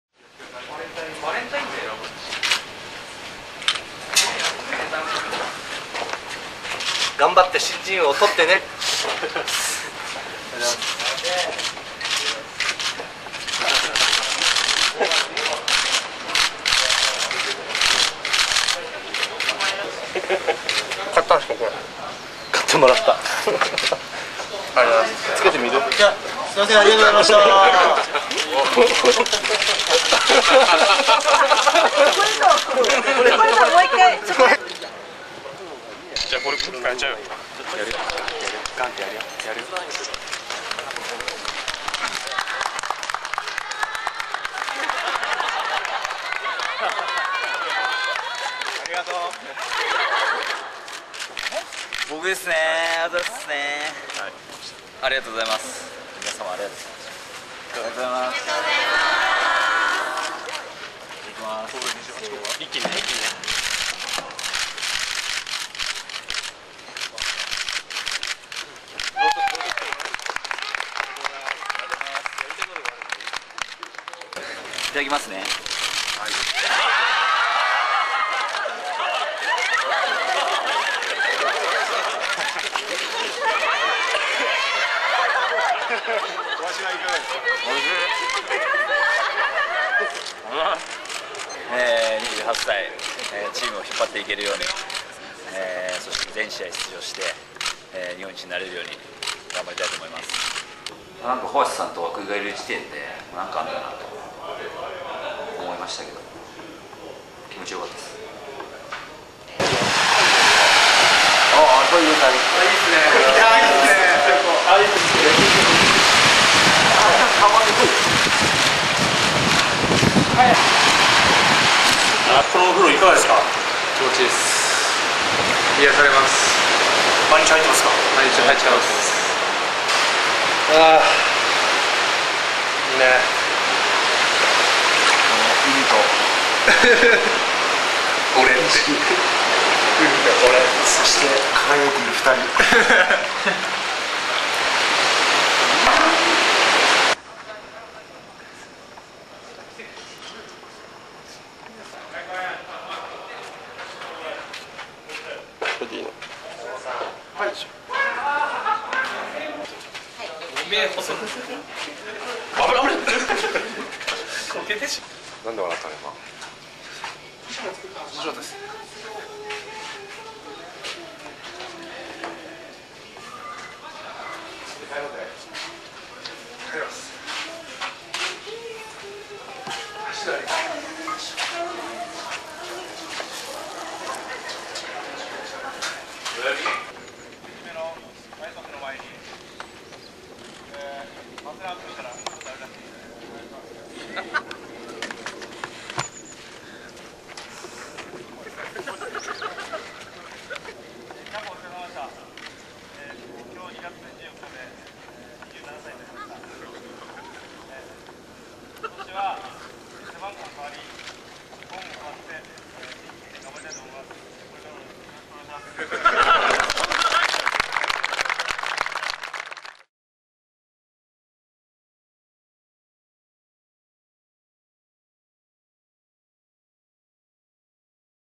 2011南郷春季キャンプメイキング映像(vol.2)
オフィシャルだけに見せてくれた選手たちの笑顔、そしてお茶目なシーンを厳選しました。

あの選手があんなことを!?
ファン必見!片岡選手がバースデーケーキに埋もれています!!

試合の時とは違った、選手の素の表情が見られると大好評のメイキング映像をお楽しみください!!